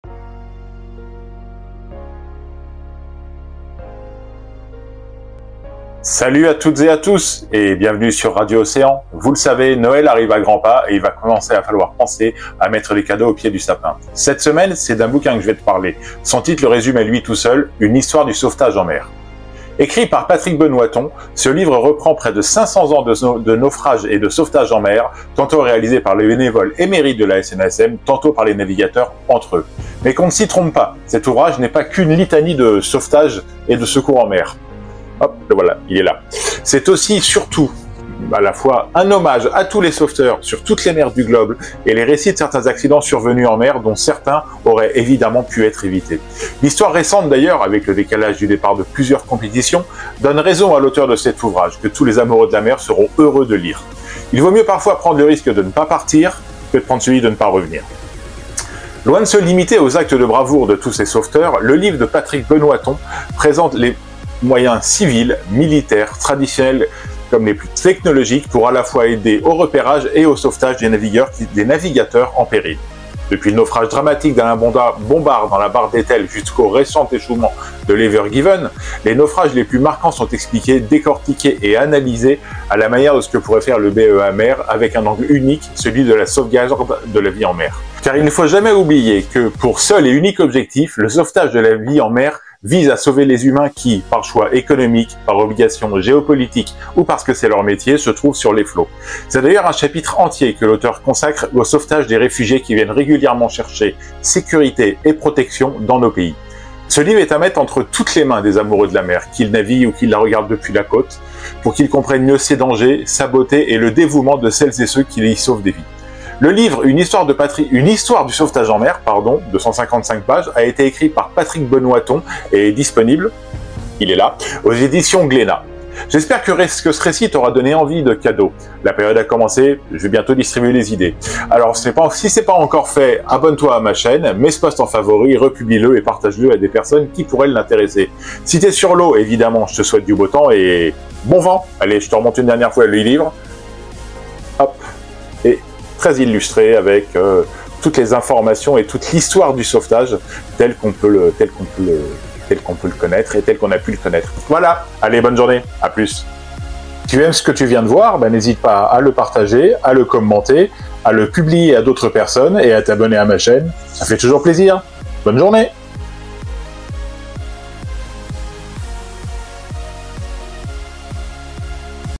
Une histoire du sauvetage en mer, un livre de Patrick Benoiton aux éditions Glenat
Voici qu'arrivent les cadeaux de Noël !

Aujourd'hui c'est d'un livre de Patrick Benoiton, Une Histoire du Sauvetage en mer, dont je voudrais te parler. Il présente, en 155 pages, l'histoire du sauvetage, ses moyens au fil du temps et explique avec moutls détails certains grands sauvetages.

C'est le livre parfait à offrir à un amoureux de la mer, des vagues et des bateaux. Ou à s'offrir à soi-même !